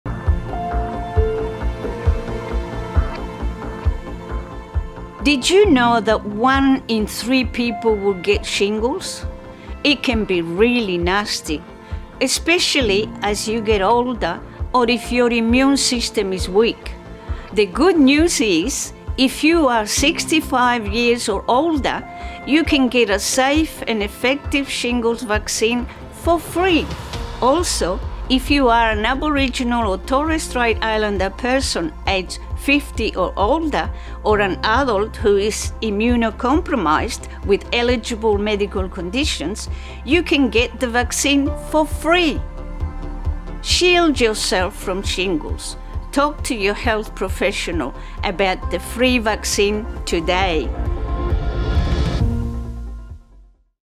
Shield yourself from shingles - Ida
Ida shares a message to those aged 65 and over about the importance of getting the free shingles vaccine.